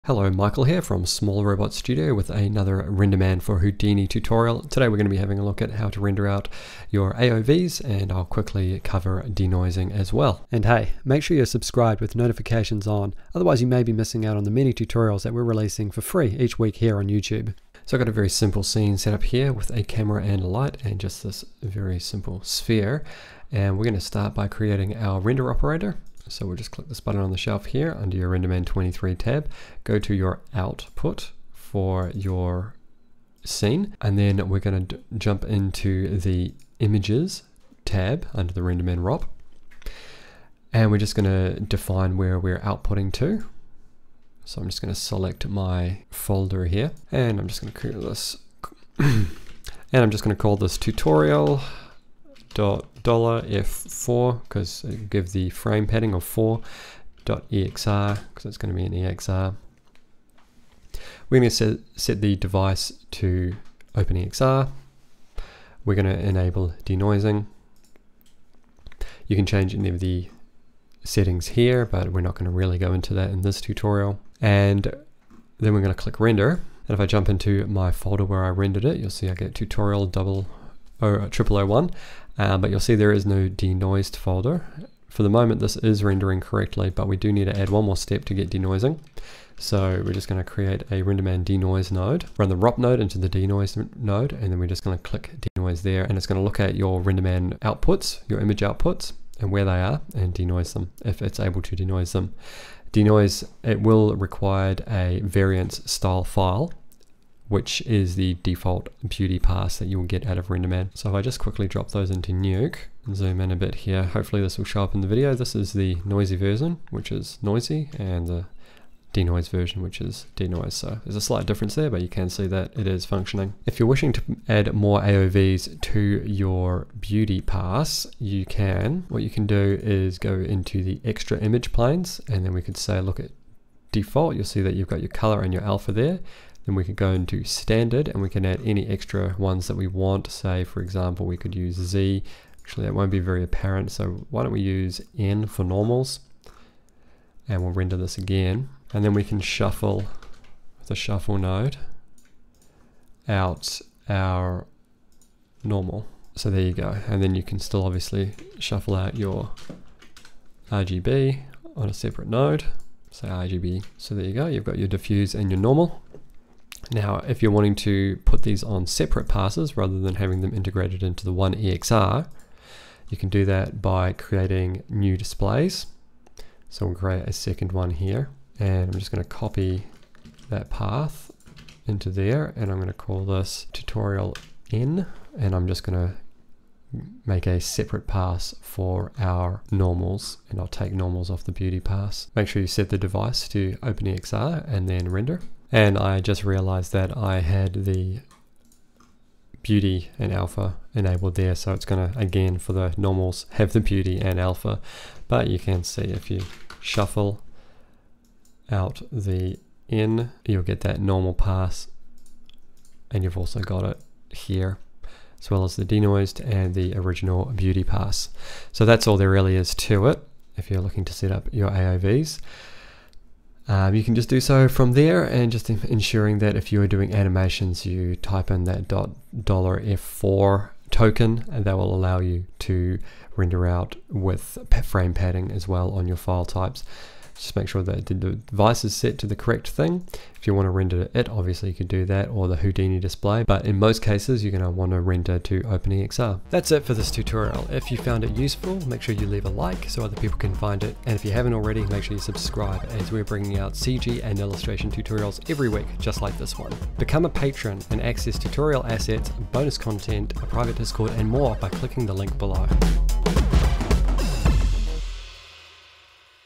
Renderman 23 AOV's and Denoising [Houdini] Tutorial
In this tutorial we'll quickly run through the steps for outputting custom AOV's and denoising.